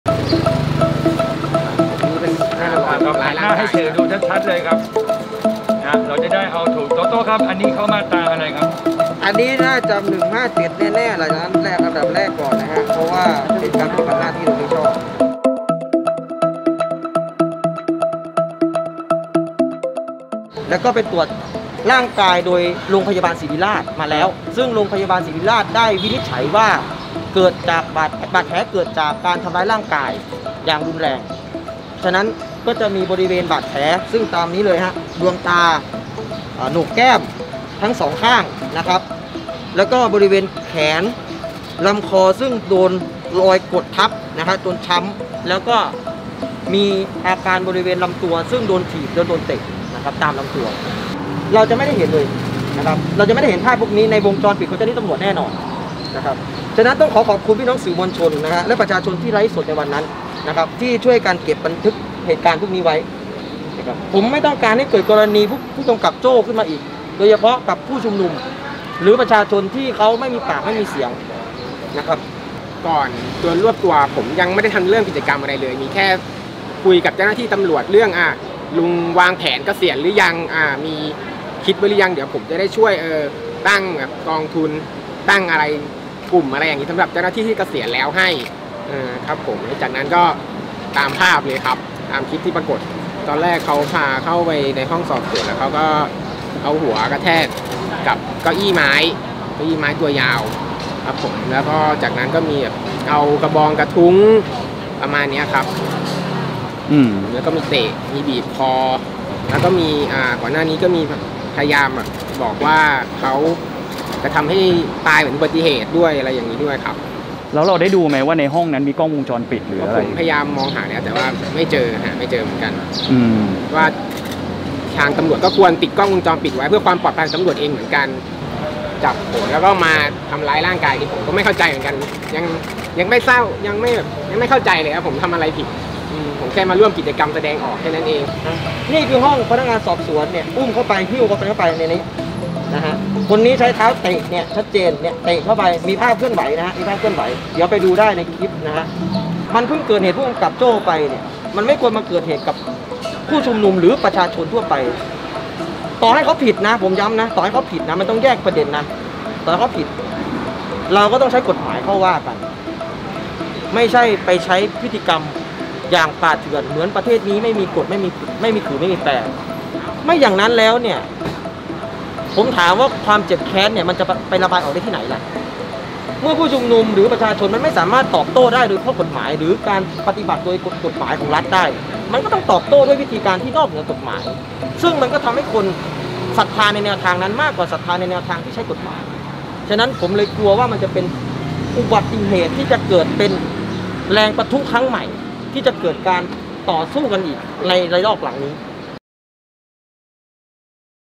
แจ้งความถูกตำรวจซ้อมหลังเหตุรำลึก 'วาฤทธิ์ สมน้อย' ที่ สน.ดินแดง
วันนี้ (1 พ.ย. 64) อรรถสิทธิ์ นุสสะ พร้อมด้วยทนาย และปิยรัฐ จงเทพ (โตโต้ We Volunteer) เข้าแจ้งความที่สถานีตำรวจดินแดง หลังจากอรรถสิทธิ์ถูกเจ้าหน้าที่ตำรวจทำร้ายร่างกายและคาดว่ามีการซ้อมทรมานในห้องสืบสวนเมื่อวันที่ 29 ตุลาคมที่ผ่านมา ซึ่งอรรถสิทธิ์เป็นหนึ่งในผู้ที่เข้าร่วมกิจกรรมไว้อาลัย “วาฤทธิ์ สมน้อย” เยาวชนอายุ 15 ปี ที่ถูกยิงเสียชีวิตหน้า สน. ดินแดงก่อนหน้านี้

"ผมไม่ต้องการให้เกิดกรณี 'ผู้กำกับโจ้' ขึ้นมาอีก โดยเฉพาะกับผู้ชุมนุมหรือประชาชนที่เขาไม่มีปากมีเสียง" ปิยรัฐ จงเทพ แกนนำ WeVo กล่าว
อ่านต่อที่